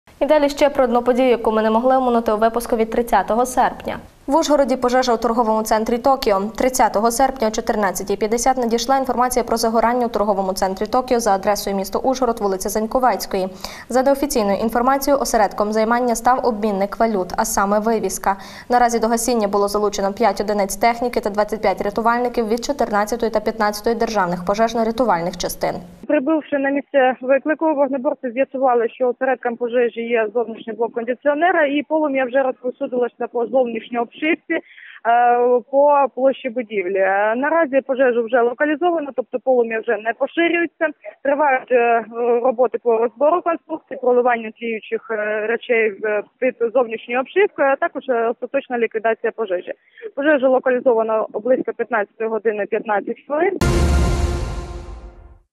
Пожежа в торговому центрі
В Ужгороді горів торговий центр “Токіо”. Пожежа сталася близько 14:50 на зовнішній стороні будівлі, на першому і другому поверхах. Щоб її погасити, задіяли тридцятьох рятувальників. Через годину пожежу ліквідували.